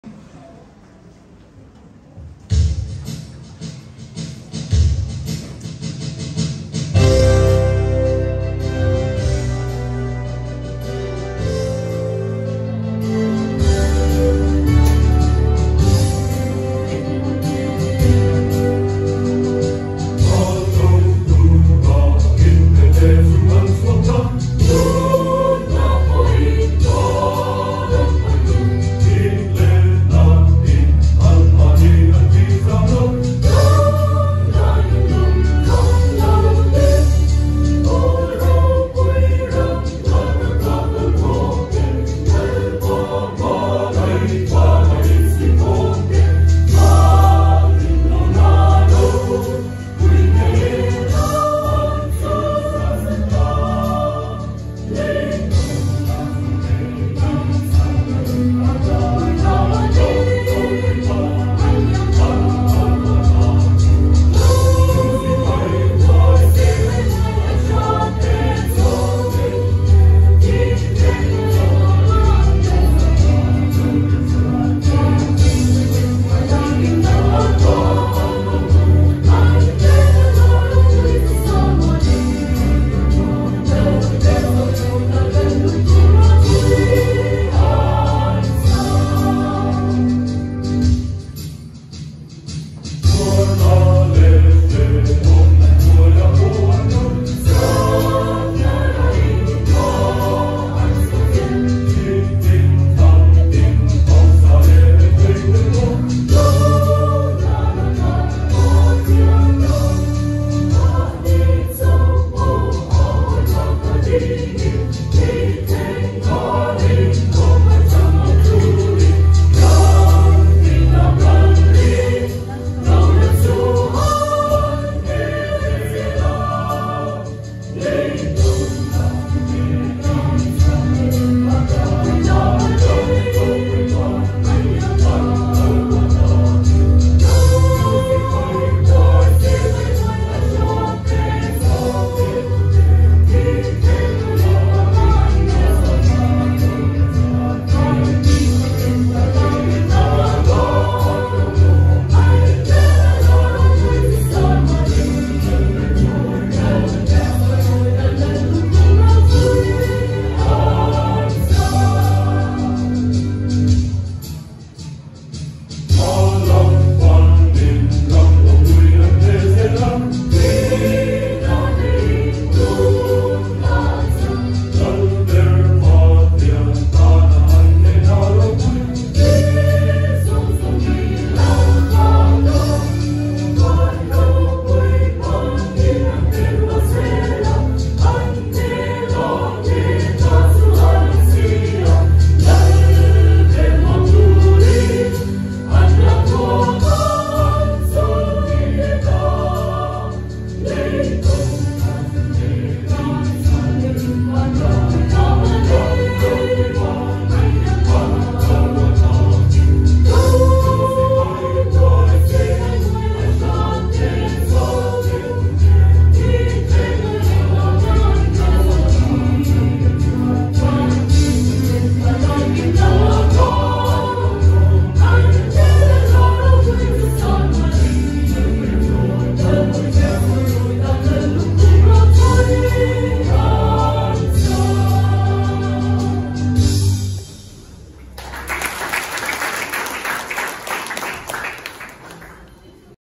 KANAN CORPS SAY : RALDO TURA INPE TE CHU AN CHHUAK TA
Local Officer Retirement Service : Kanan Corps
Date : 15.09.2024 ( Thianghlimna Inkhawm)

Pu D Khana, Corps Sergeant Major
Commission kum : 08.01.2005

Pi Biaksailovi, Assistant Home League Secretary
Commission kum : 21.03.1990

PI Kapzami, Home League Welcome Sergeant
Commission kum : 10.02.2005